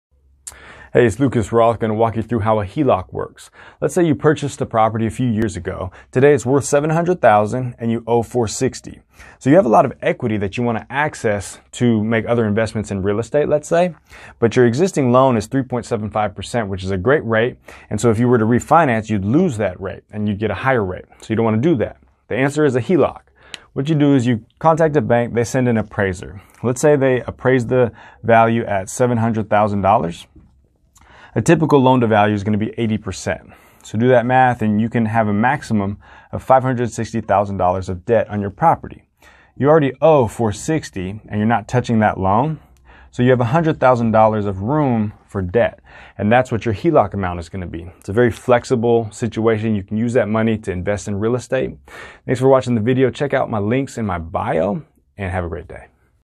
HELOC Example (60 secs)
Here's how HELOCs work (Home Equity Line of Credit) in only 60 seconds.  This is a tool you use when:
1) You want to access equity from your home
2) You don't want to refinance out of your current mortgage (because your mortgage rate is lower than the rate currently available)
3) You may not need the extra money for a while - but you want the money available to you